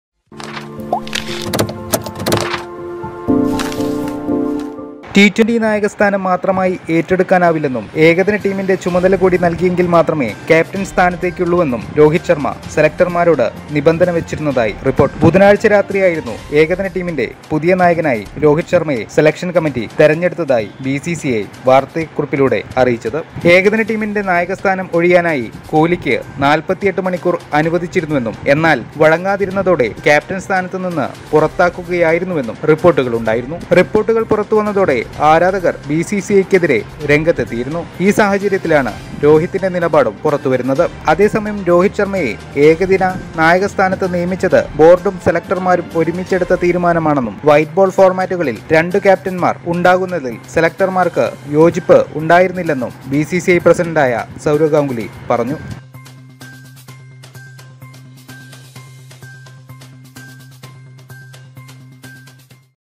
രോഹിത് ഏകദിന ക്യാപ്‌റ്റൻ സ്ഥാനം ആവശ്യപ്പെട്ടു?
ബുധനാഴ്ച രാത്രിയായിരുന്നു ഏകദിന ടീമിന്റെ പുതിയ നായകനായി രോഹിത് ശര്‍മയെ സെലക്ഷന്‍ കമ്മിറ്റി തിരഞ്ഞെടുത്തതായി ബിസിസിഐ വാർത്താക്കുറിപ്പിലൂടെ അറിയിച്ചത്.